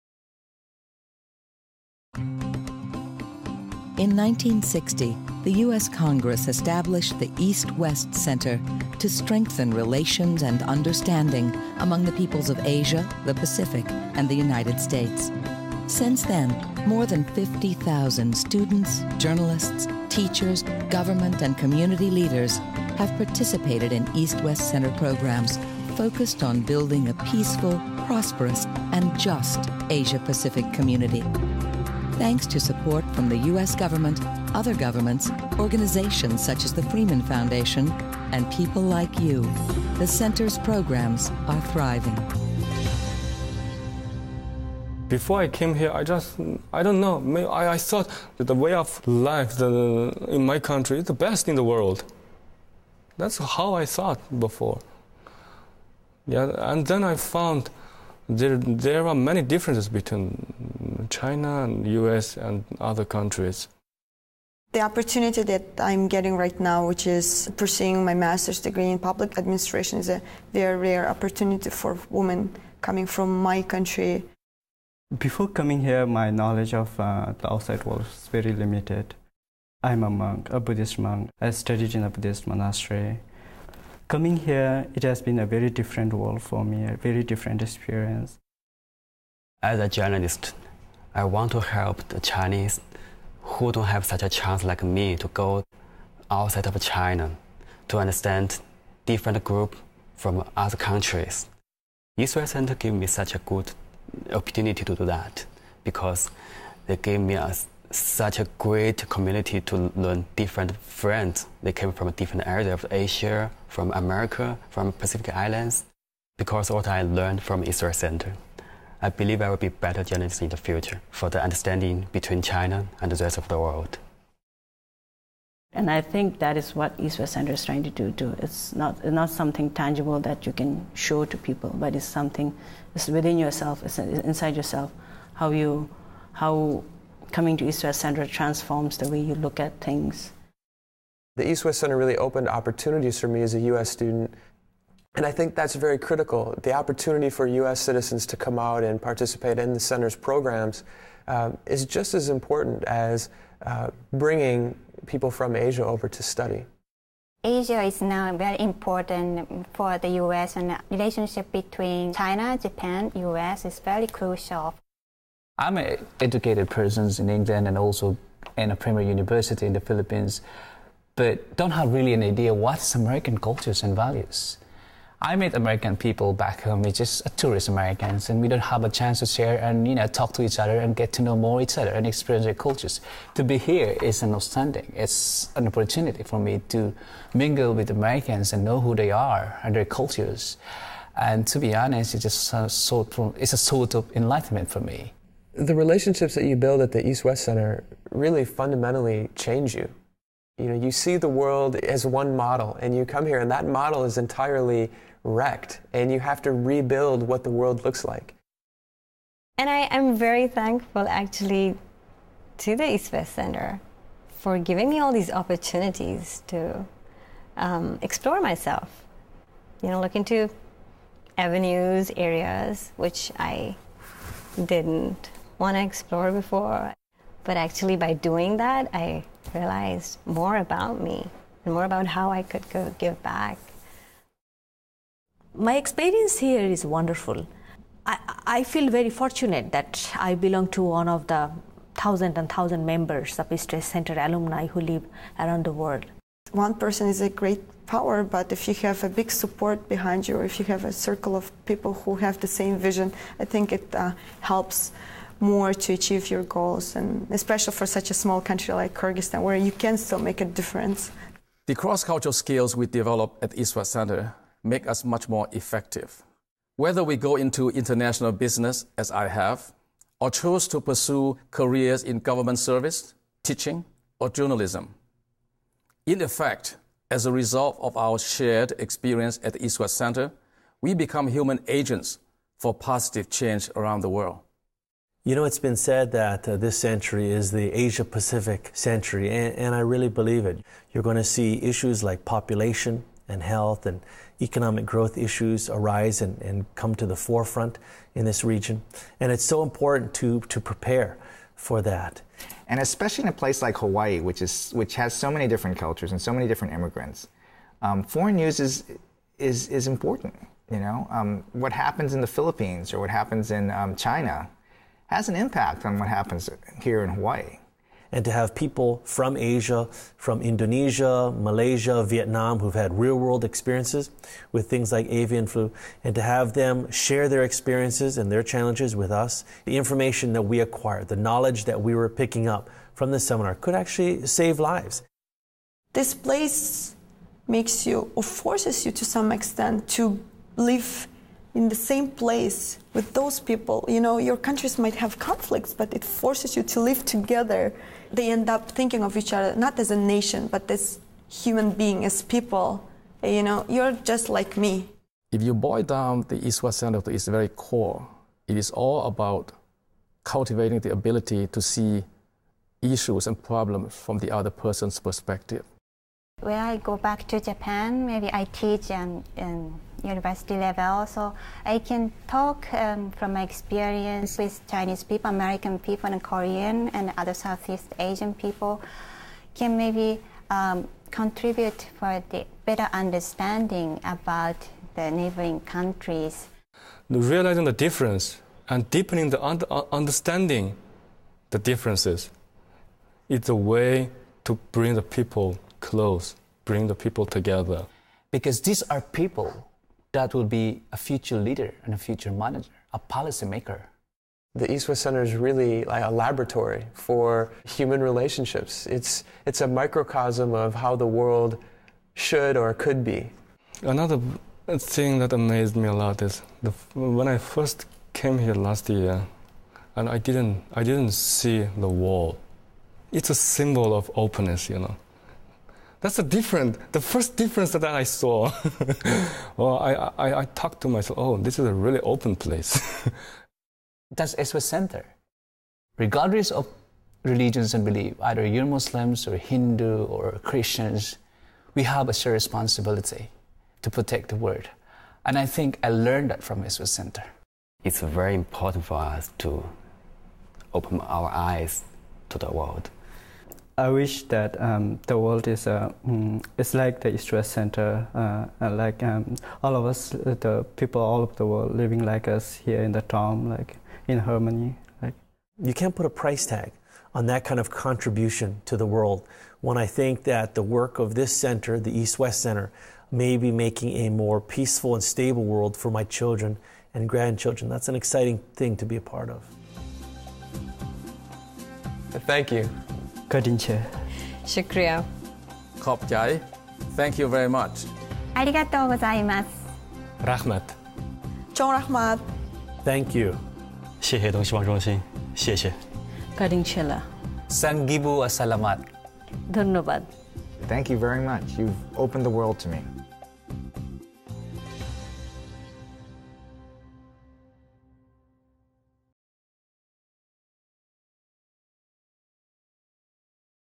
The East-West Center Experience
East-West Center students describe how the Center has enriched their educational experience and their lives.